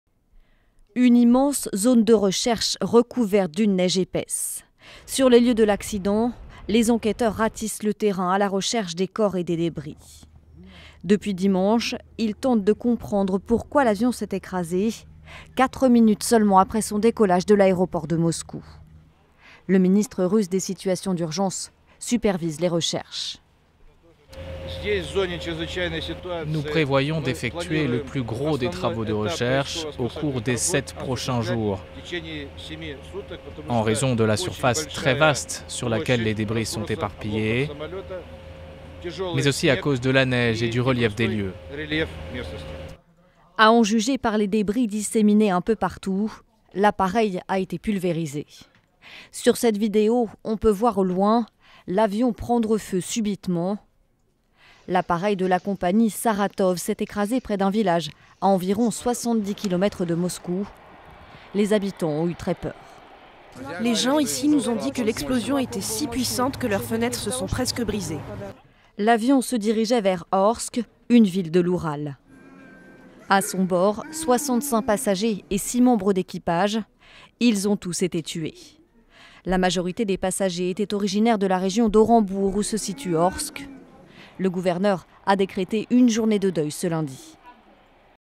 Russie : la neige complique l''enquête sur le crash de l''avion Saratov
Une immense zone de recherche recouverte d''une neige épaisse sur les lieux de l''accident,les enquêteurs ratissent le terrainà la recherche des corpset des débris. Depuis dimanche, ils tentent de comprendre pourquoi l''avion s''est écrasé,4 minutes seulement après son décollage de l''aéroport de Moscou. Le ministre russe des situations d''urgence supervise les recherches. SOT Vladimir Puchkov, Ministre russe des situations d''urgence  "Nous prévoyons d''effectuer le plus gros des travaux de recherches au cours des sept prochains jours, en raison de la surface très vaste sur laquelle les débris sont éparpillés, de la neige et du relief des lieux"A en juger par les débris disséminés un peu partout,l''appareil a visiblement été pulvérisé.Sur cette vidéo on peut voir au loin,l''avion prendre feu subitement. L''appareil de la compagnie Saratov s''est écrasé près d''un village, à environ 70 km de Moscou. Les habitants ont eu très peur. SOT témoins"Les gens  ici nous ont dit que l''explosion était si puissante que leurs fenêtres se sont presque brisées."L''avion se dirigeait vers Orsk, une ville de l''Oural.A son bord, 65 passagers et  6 membres d''équipage.Ils ont tous été tués.   La majorité des passagers étaient originaires de la région d''Orenbourg où se situe Orsk...Le gouverneur a décrété une journée de deuil lundi.